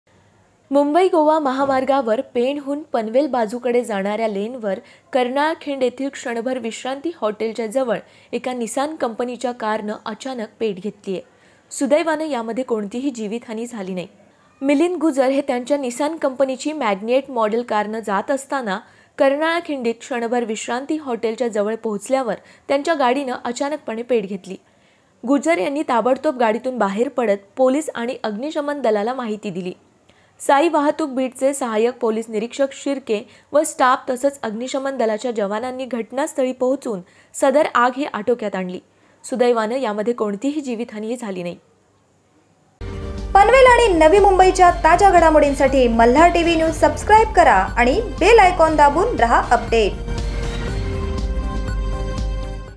मुंबई गोवा महामार्गावर कर्नाळा खिंडीत गाडीने घेतला अचानक पेट !!!
मुंबई गोवा महामार्गावर पेणहुन पनवेल बाजूकडे जाणारे लेन वर कर्नाळा खिंड  क्षणभर विश्रांती हॉटेलचे जवळ एक निसान कंपनीची कारने अचानक पेट घेतली. सुदैवाने यामध्ये कोणतीही जीवित हानी झाली नाही. मिलिंद गुजर हे त्यांच्या निसान कंपनीची मॅग्निएट मॉडेल कारने जात असताना कर्नाळा खिंड क्षणभर विश्रांती हॉटेलचे जवळ पोहोचल्यावर त्यांच्या गाडीने अचानक पेट घेतली. गुजर यांनी ताबडतोब गाडीतून बाहेर पडत पोलीस आणि अग्निशमन दलाला याची माहिती दिली. साई वाहतूक बिट चे सहाय्य्क पोलीस निरीक्षक शिर्के व स्टाफ तसेच अग्निशमन दलाच्या जवानांनी घटनास्थळी पोहचुन सदर आग आटोक्यात आणली. सुदैवाने यामध्ये कोणतीही जीवित हानी झालेली नाही.